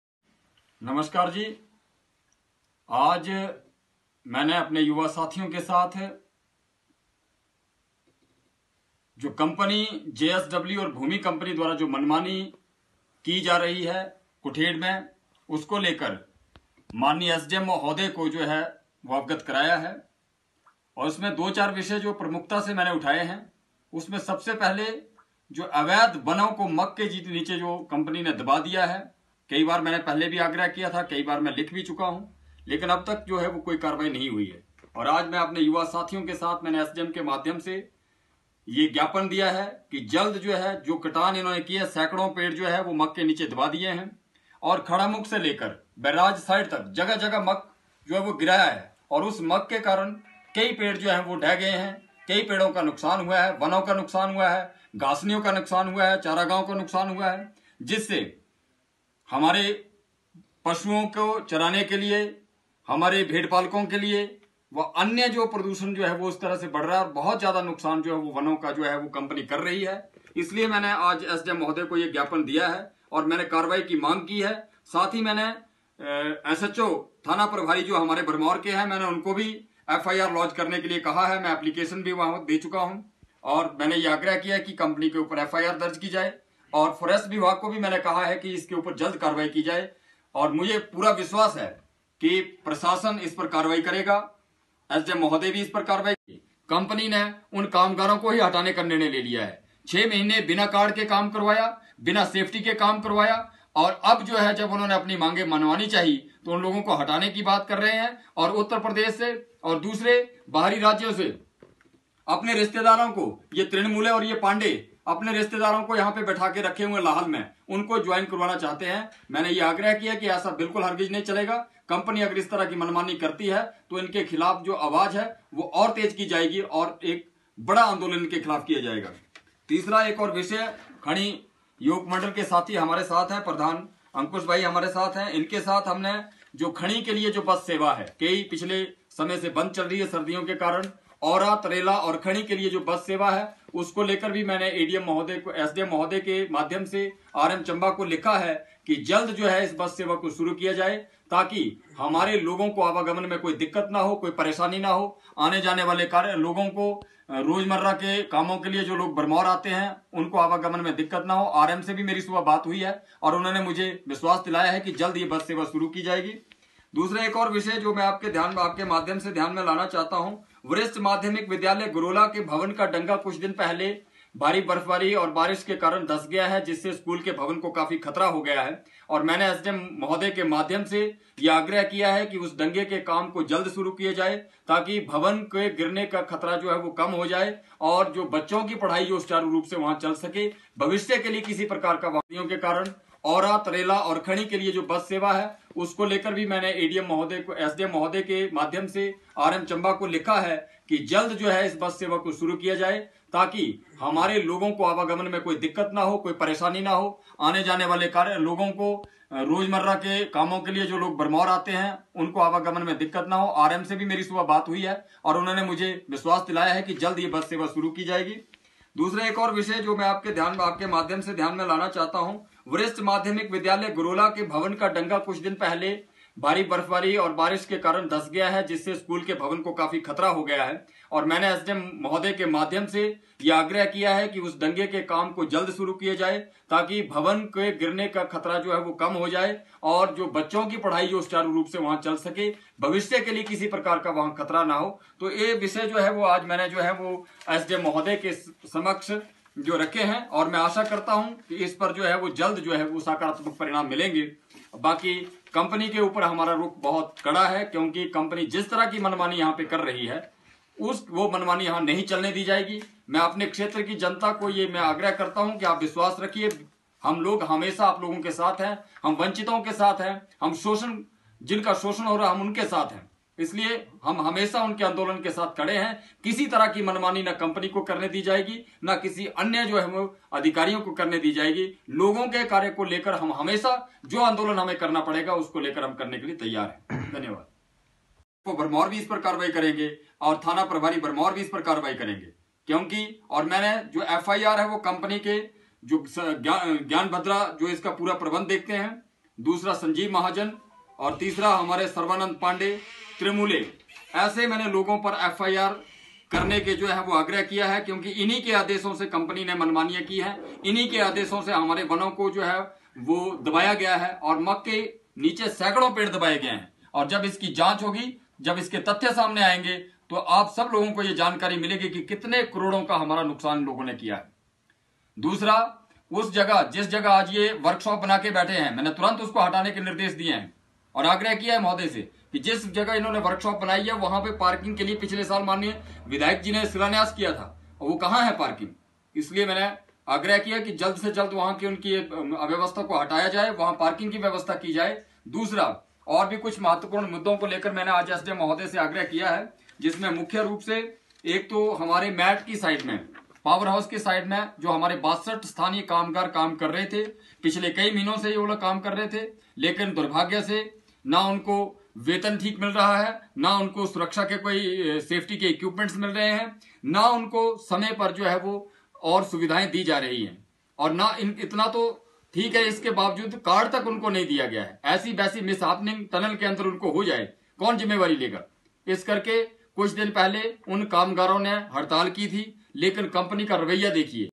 कुठेड़ प्रोजेक्ट में बरती जा रही अनियमताओं के लिए हो कानूनी कार्रवाई .... अनिल ढकोग Tribal Today Tv
कुठेड़ हाइड्रो पावर प्रोजेक्ट में लगातार बेटी जा रही अनियमताओं की शिकायत सोमवार को जिला परिषद सदस्य अनिल कुमार ने एस डी एम भरमौर से की उन्होंने अपनी शिकायत में कहा कि इस परियोजना के निर्माण कार्य में भारी अनियमताएं बरती जा रही है लिहाजा जे एस डवल्यू तथा भूमि कम्पनी के अधिकारियों पर कानूनी कार्यवाही अमल में लाई जानी चाहिए।